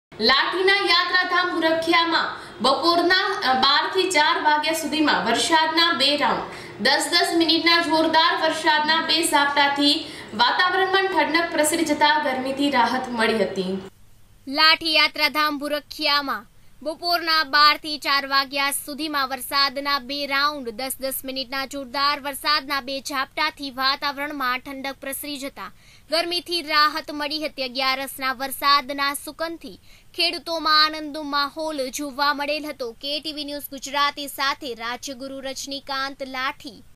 લાઠીના યાત્રધામ ભુરખીયા ભુરખીયામા બપોરના બારથી ચાર વાગ્યા સુધીમા વરસાદ
લાઠીના યાત્રધામ ભુરખીયા ભુરખીયામા બપોરના બારથી ચાર વાગ્યા સુધીમા વરસાદના બે રાઉન્ડ.દસ-દસ મીનીટના જોરદાર વરસાદના બે ઝાપટાથી વાતારણમા ઠંડક પ્રસરી જતા ગરમીથી રાહત મળી હતી.